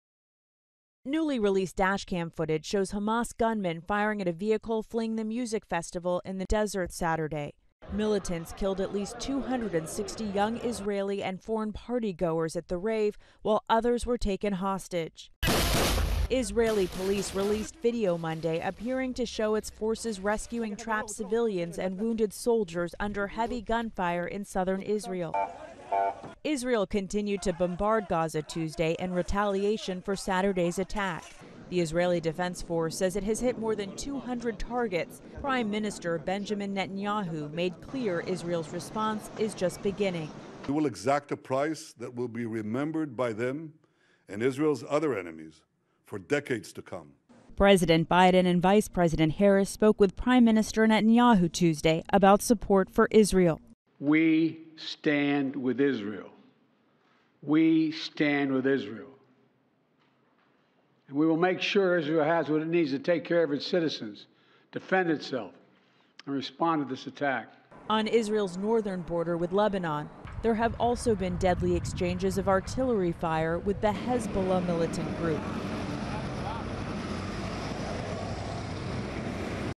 President Biden condemns Hamas
WASHINGTON (AP) — President Joe Biden on Tuesday condemned the militant group Hamas for “sheer evil” for its shocking multipronged attack on Israel launched from the Gaza Strip that has killed hundreds of civilians, including at least 14 American citizens.